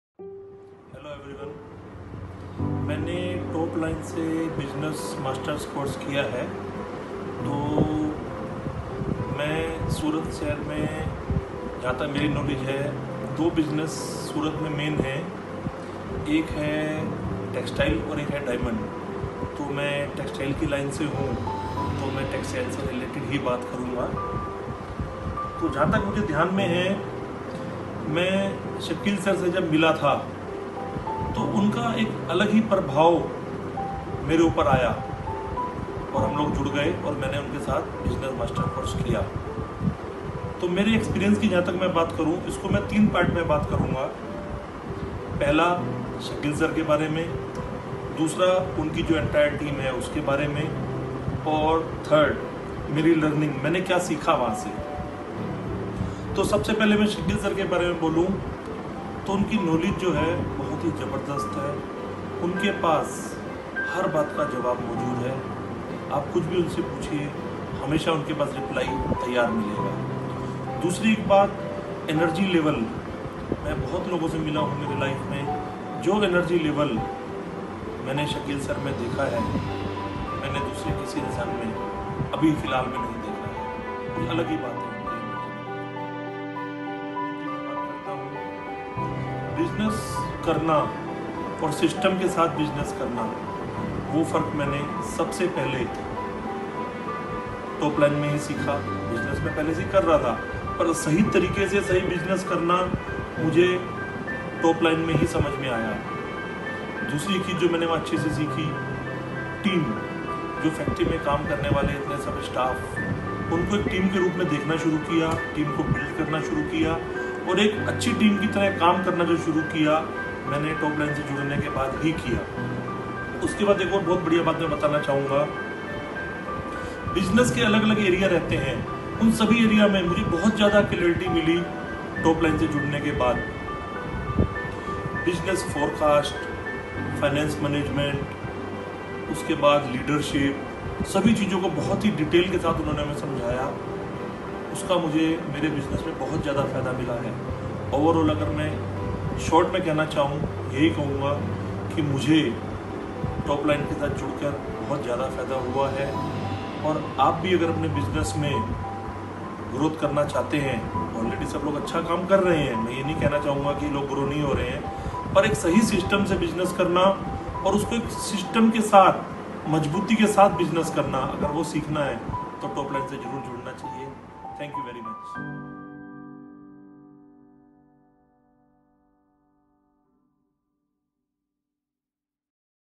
सूरत के एक सफल बिज़नेसमेन की रोचक बात - सुनिए उनकी जुबानी
सचिन GIDC स्थित क्रिष्णा एम्ब्रोइडरी,जो की सूरत की बेहद जानीमानी एम्ब्रोइडरी क्रिएशन की कंपनीओ मे से एक है, और जिनके लीडर Mr. राजेश भगतजी जो खुद भी एक अच्छी पर्सनालिटी, अच्छी कम्युनिकेशन और जबरदस्त एंट्रोपेर्नोरसीप स्किल के धनि हे।
आइये हम जानते है जब इतने अमेजिंग, काबिल बिज़नेसमेन जब टोपलाइन बिज़नेस डेवलपमेंट सर्विसिस के प्रोग्राम अटेन्ड करते है तब उनका एक्सपीरियंस केसा रहता है।
हम राजेश भगतजी के इस अमेजिंग फीडबैक एवं प्रभावशाली विवरण के लिए उनके आभारी है।